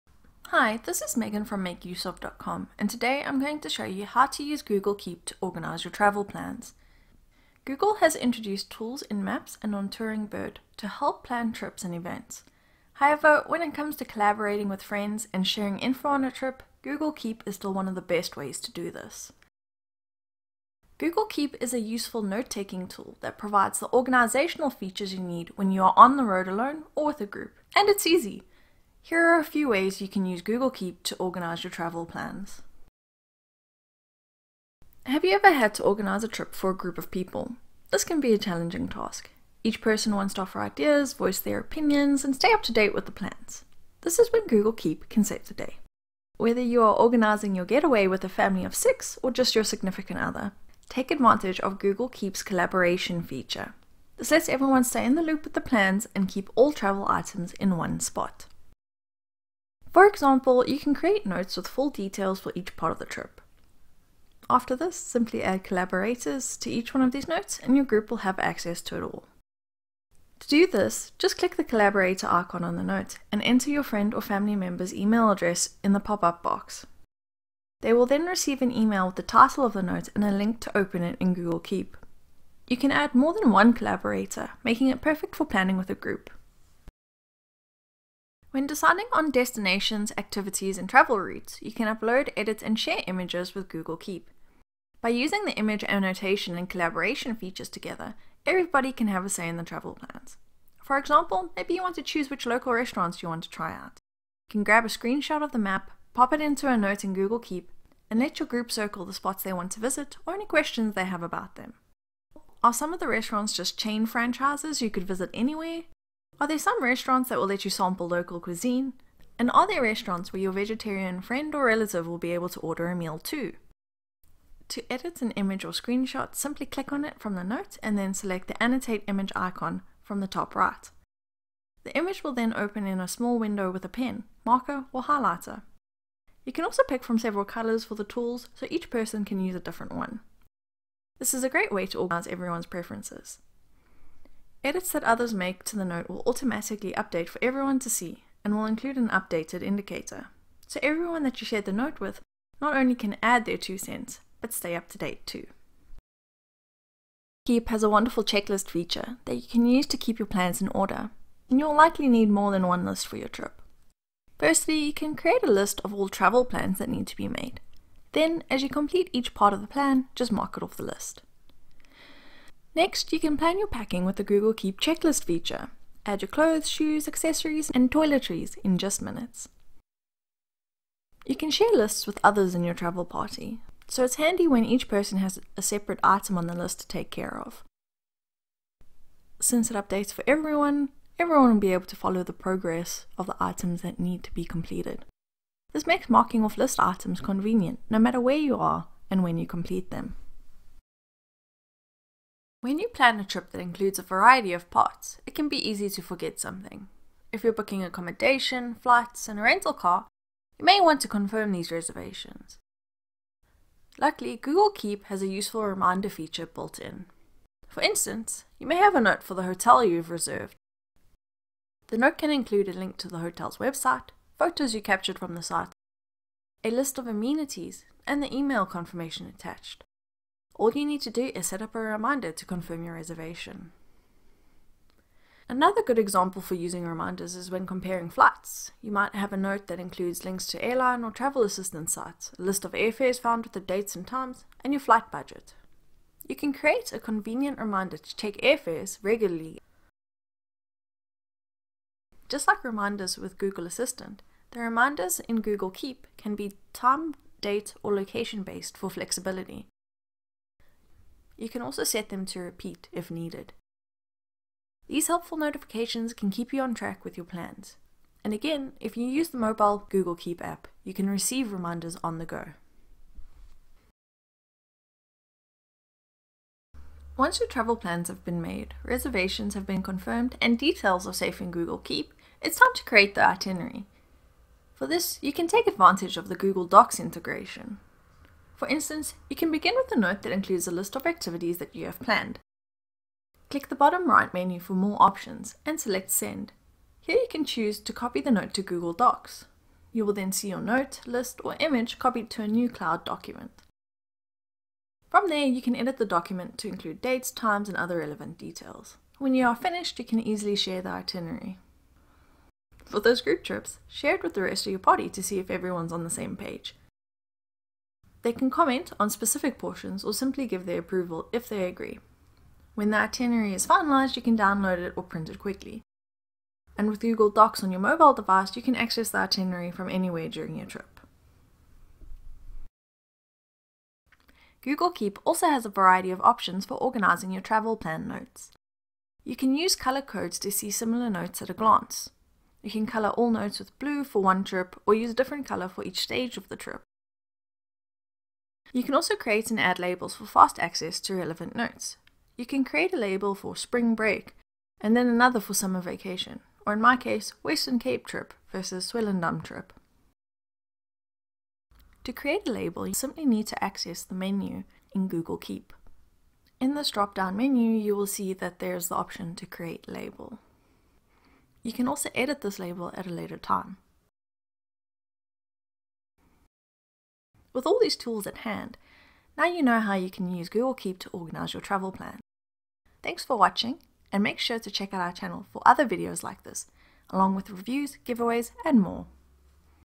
How to Organize Your Travel Plans with Google Keep
You need hotel reservations, flights, and a list of activities. When you make travel plans keeping it all together is essential. A humble tool packs a few powerful features you shouldn’t overlook for your next trip.

Google Keep is a useful note-taking tool that provides the organizational features you need when you are on the road alone or with a group. And it’s easy!